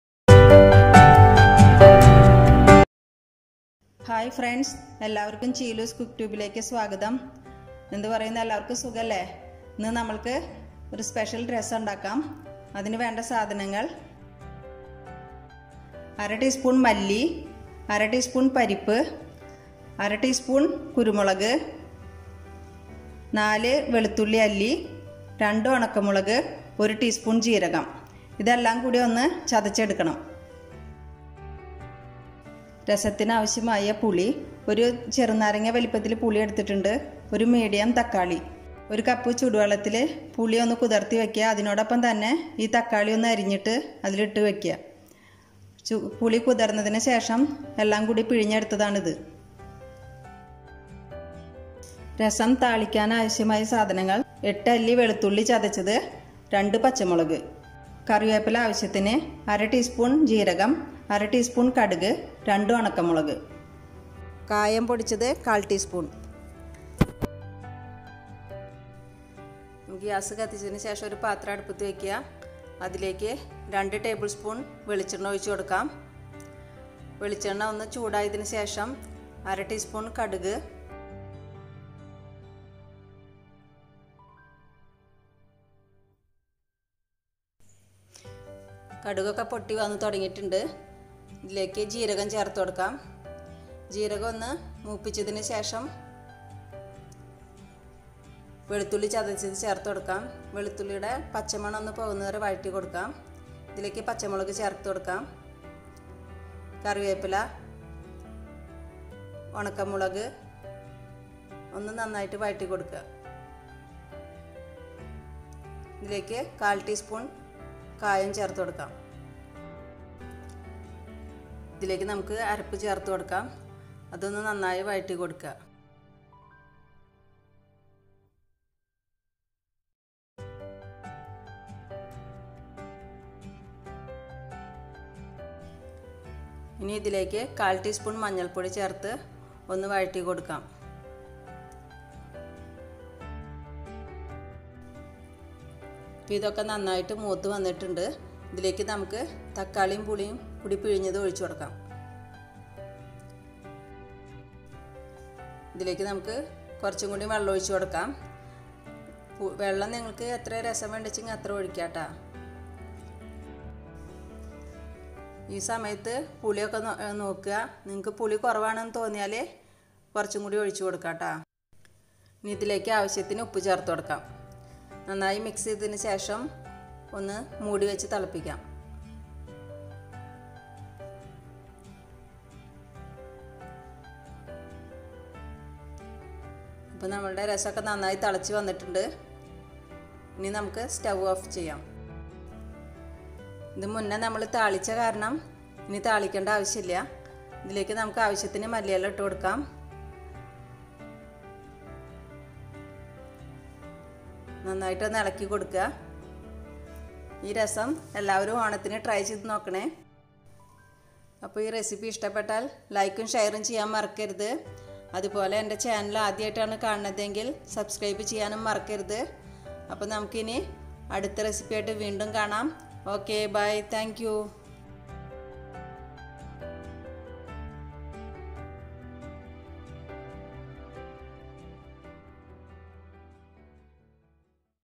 Special Spicy Rasam Recipe
INGREDIENTS
1/2 tsp Coriander
1/2 tsp toor dal
1/2 tsp pepper corns
4 garlic cloves
2 dry red chili
1 tsp cumin seeds
Tamarind  & 1 tomato  chopped (soaked in 1 cup of hot water)

FOR SEASONING
2 tbl spn Coconut oil
1/2 tsp Mustard
1/2 tsp Cumin seeds
2 Dry red chili
2 Green chili
1/4 tsp Turmeric powder
8 Garlic chopped
1/4 tsp Asafoetida powder